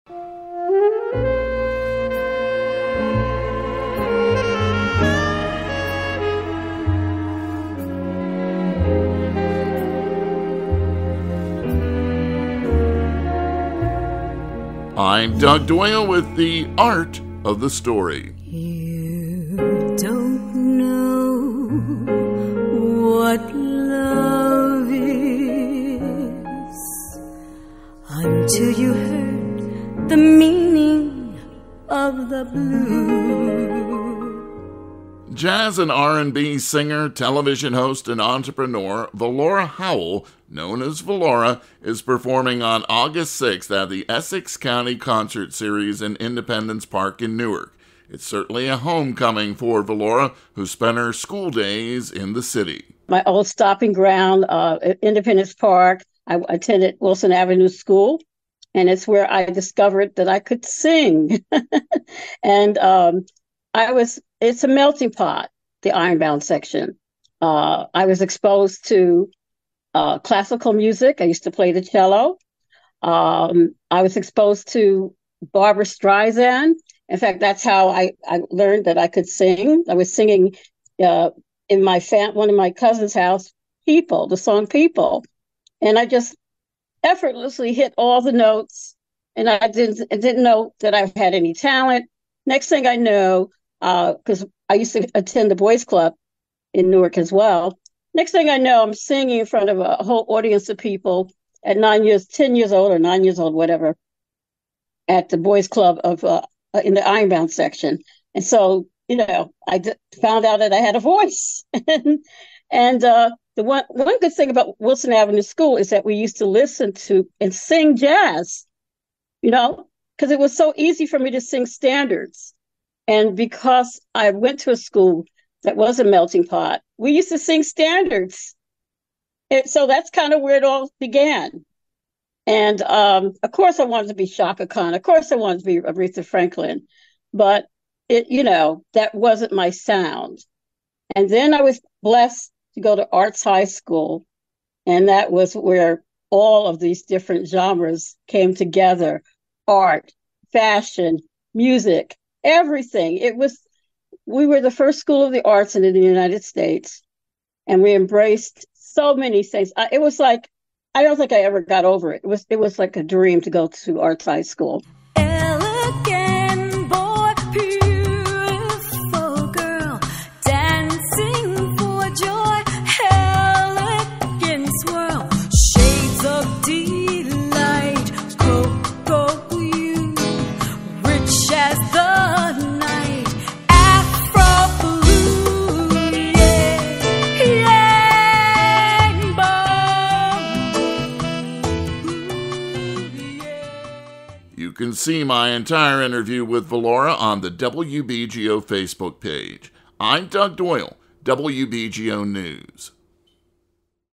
Jazz and R & B singer and TV host Volora returns to Newark for a special concert on August 6
On this episode of The Art of the Story, Jazz and R & B singer and TV host Volora talks about her school days in Newark